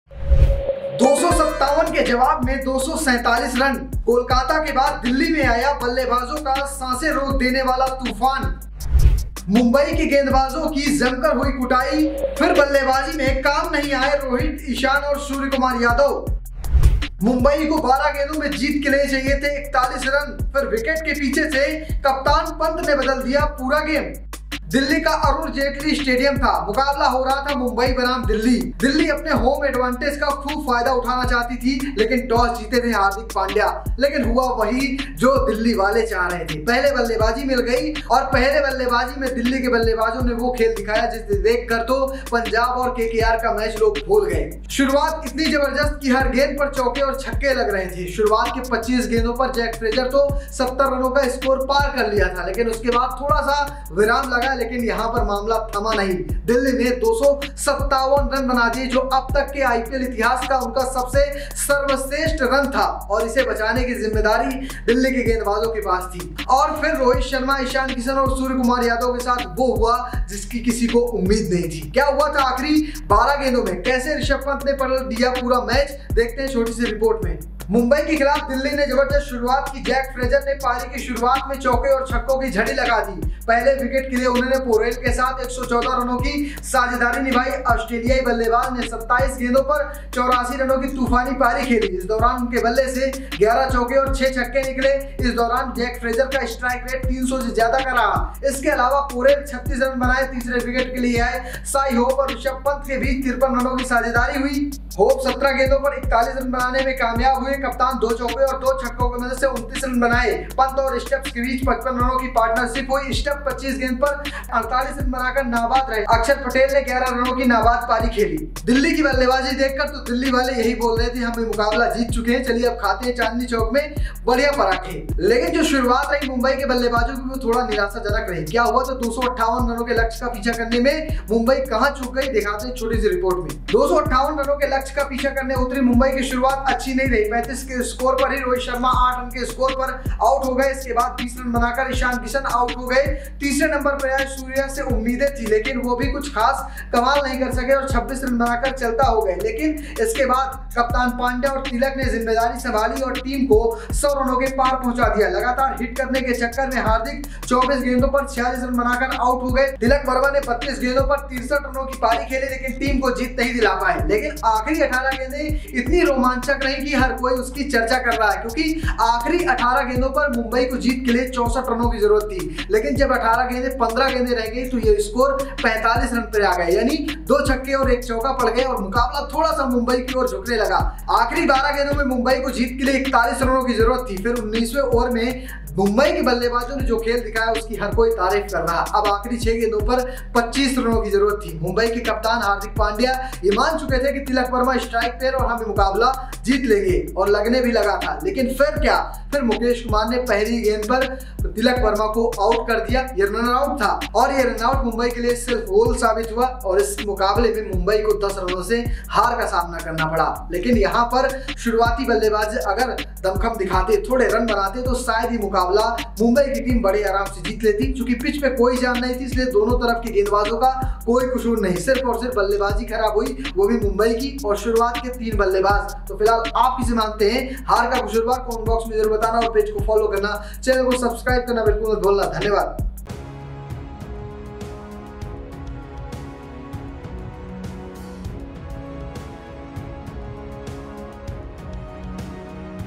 DC vs MI Highlights 2024: Delhi ने Mumbai को 10 रनों से हराया, Playoff के लिए बदल गए सभी समीकरण!
DC vs MI highlights 2024: आईपीएल 2024 का 43वां मैच दिल्ली कैपिटल्स और मुंबई इंडियंस (Delhi Capitals vs Mumbai Indians) के बीच खेला जा रहा है। मुंबई (Mumbai) ने टॉस जीतकर पहले गेंदबाजी का फैसला लिया है। दिल्ली (Delhi) ने मुंबई के खिलाफ 20 ओवर में चार विकेट खोकर 257 रन बनाए। जवाब में मुंबई ने 20 ओवर में नौ विकेट गंवाकर 247 रन बनाए। इसी के साथ दिल्ली ने ये मुकाबला 10 रन से जीत लिया।